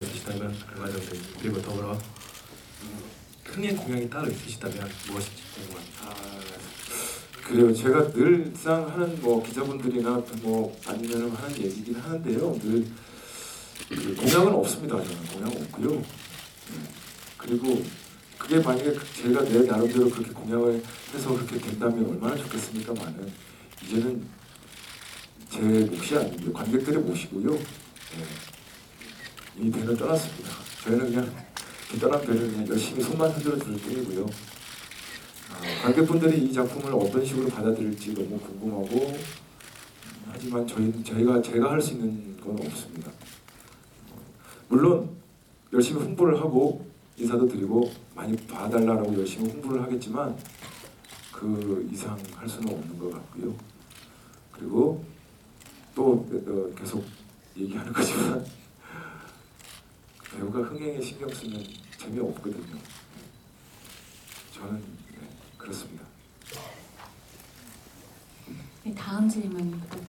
황정민 "배우는 흥행에 연연하지 않는다." [영화] 히말라야 시사회 현장 (2015.12.7)
2015년 12월 7일(월) 서울 왕십리CGV에서 열린 영화 '히말라야' 시사회&기자간담회현장. 감독: 이석훈 출연: 황정민,정우,조성하,김인권, 라미란,김원해 정유미,유선 [제작:JK필름 제공/배급/공동제작:CJ엔터테인먼트 홍보:흥미진진] (2015년 12월 16일 개봉/12세이상관람가) (박재환@KBS미디어)